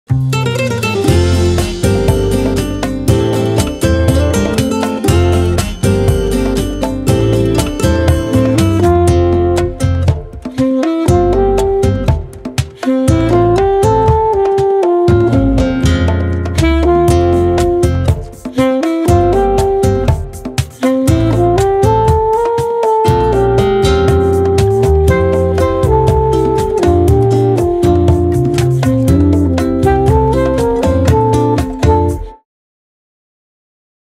【TVガイド連載／志尊淳＆「ブレークタイム」編の撮影メーキング動画☆】
TVガイドでは【志尊淳＆】を好評連載中！

「＆〇〇編」と題して、毎週テーマを変えて、俳優・志尊淳の知られざる魅力や考え方に迫っています。

今回から「＆ブレークタイム」編をお届け。多忙な志尊さんの“心休まるひととき”について聞きました！

☆最新芸能情報はインターネットTVガイドにも掲載していますので、ぜひご覧ください！